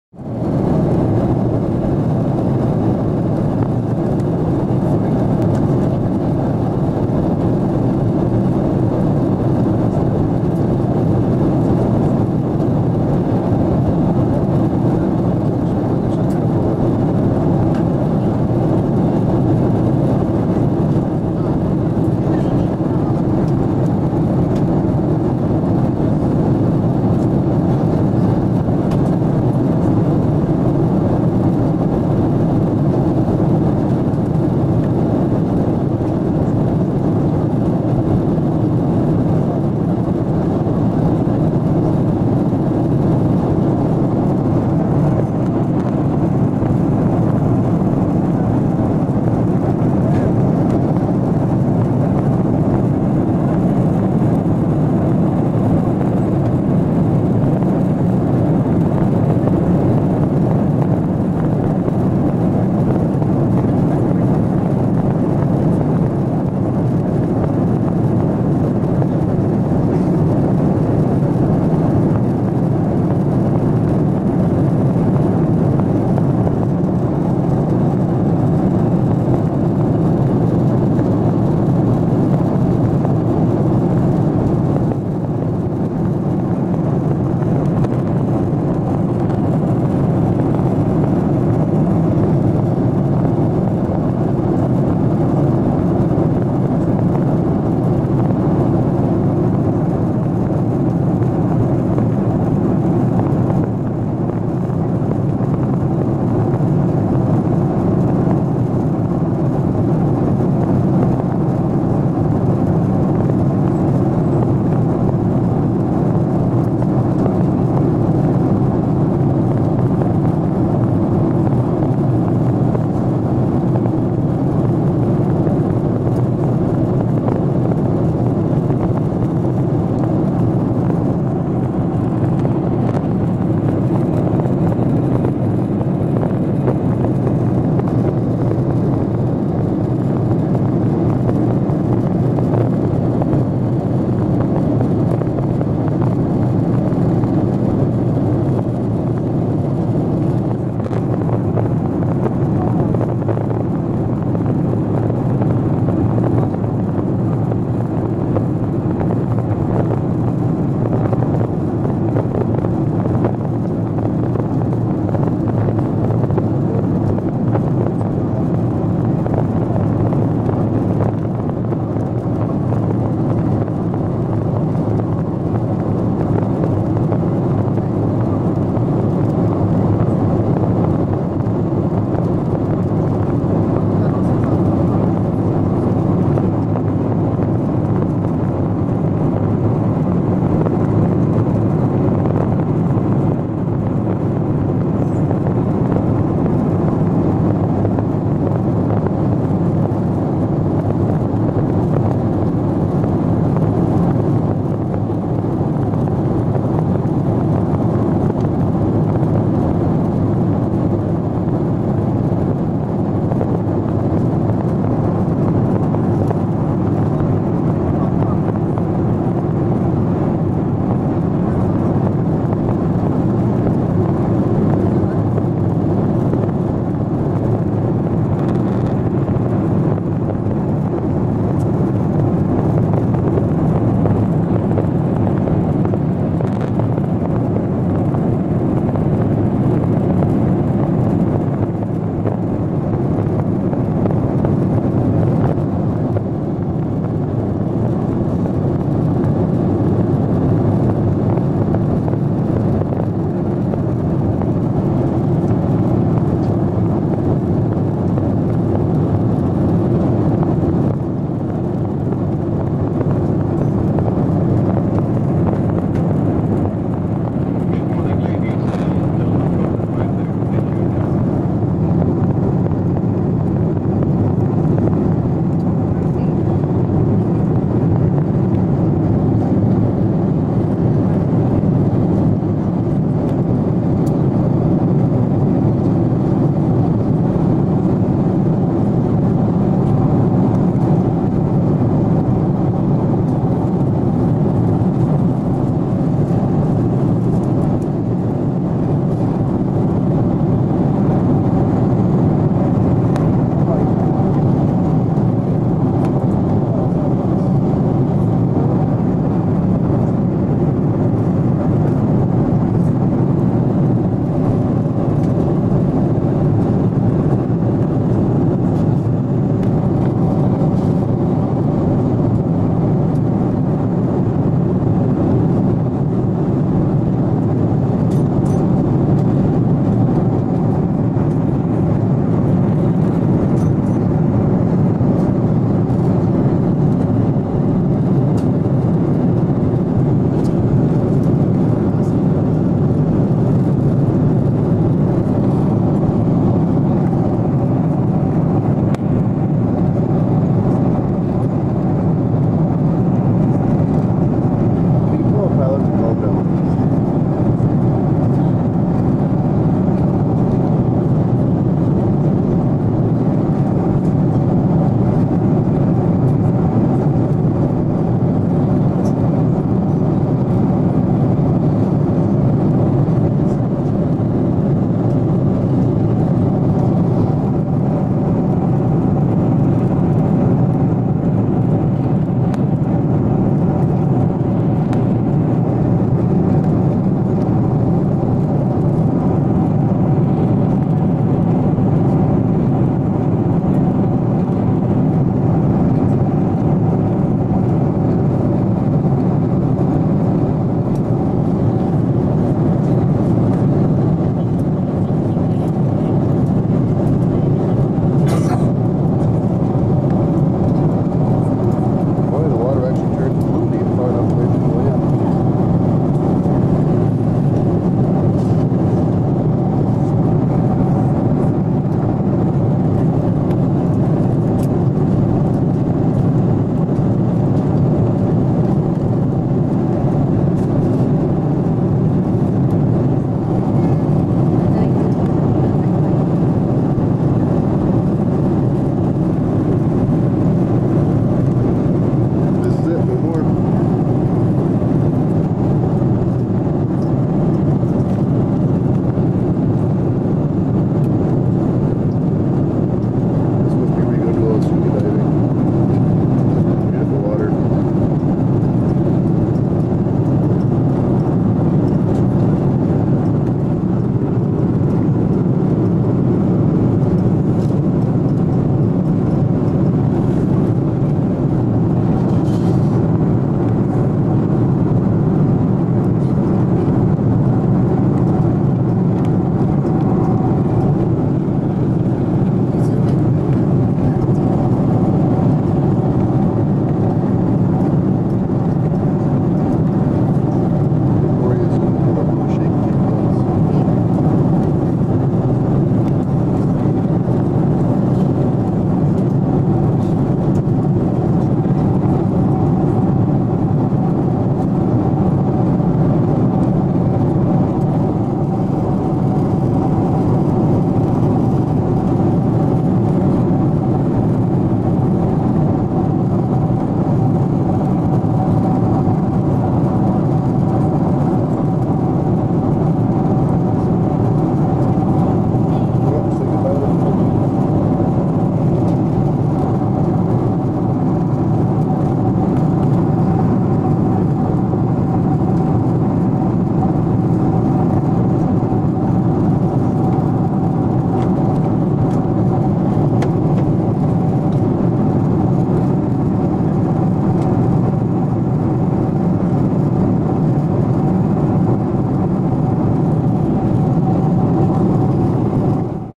Northern Philippines
Heading to Hong Kong. This is the last 10 minutes of flight prior to the South China Sea.